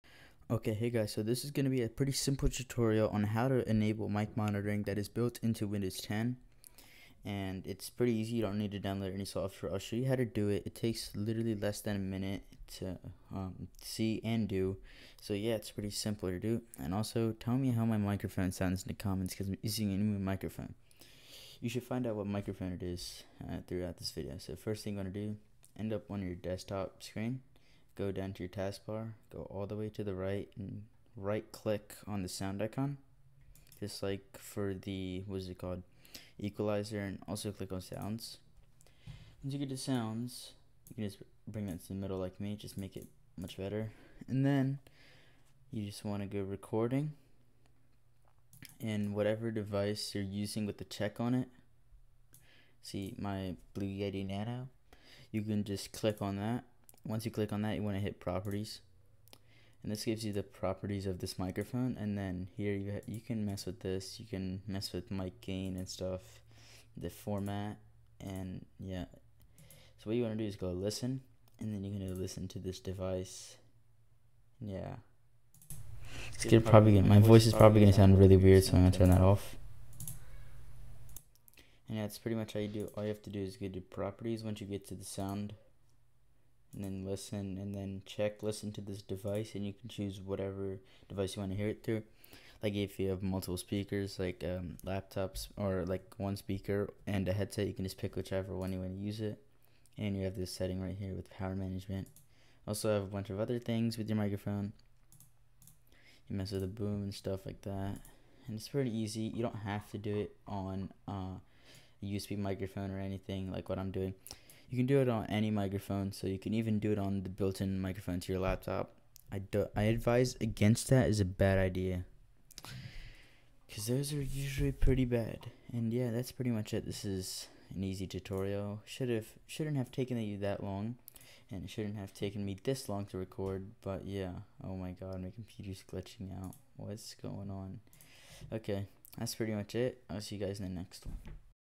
How to Enable Microphone Monitoring Built into Windows 10
Plz don't come at me for my weird desktop.
Also tell me how the microphone sounds compared to my last and previous videos.